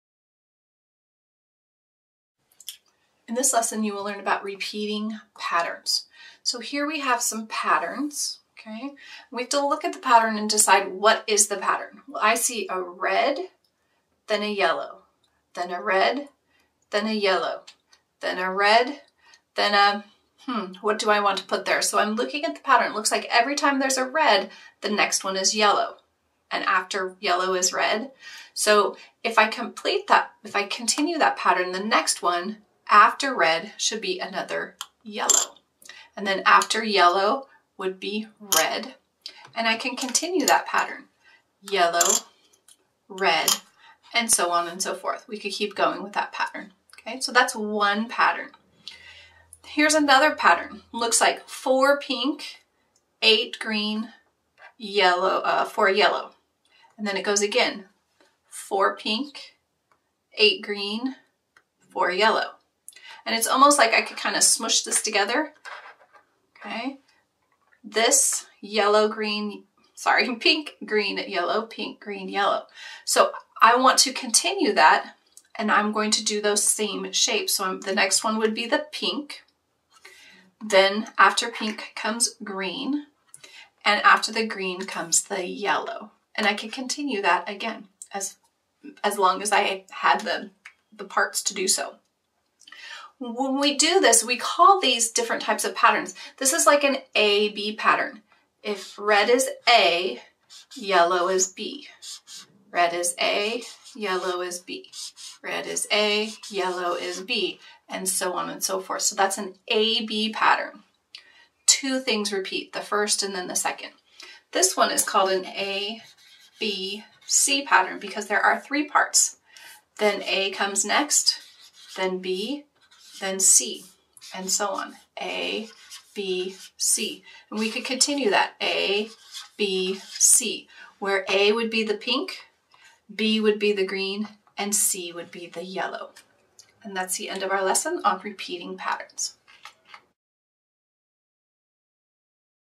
Repeating Patterns Video For Kids, Math Lesson For Kindergarten, 1st and 2nd Grade
Repeating Patterns Video For Kids, Math Lesson For Kindergarten, 1st and 2nd Grade. This lesson teaches children to recognize repeating patterns. The teachers uses objects to show this skill in a simplified manner. We find patterns in several aspects of life and here is a good way to learn to figure them out. Patterns that repeat reflect occurrences that we experience in a continuous cycle. Have fun and get more practice on the patterns worksheets found in the website above.